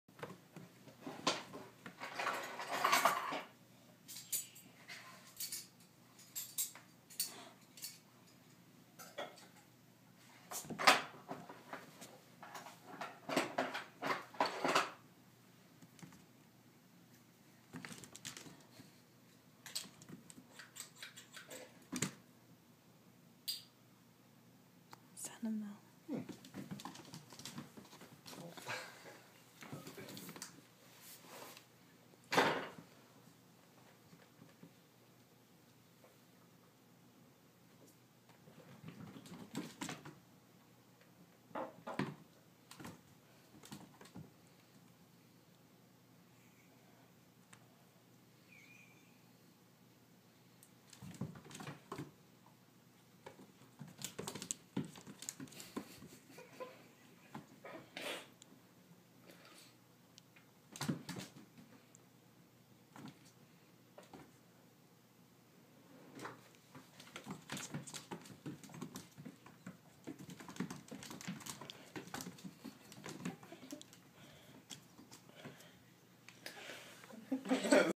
Kitten chases laser pointer
Bradshaw chases the red dot! Quick head!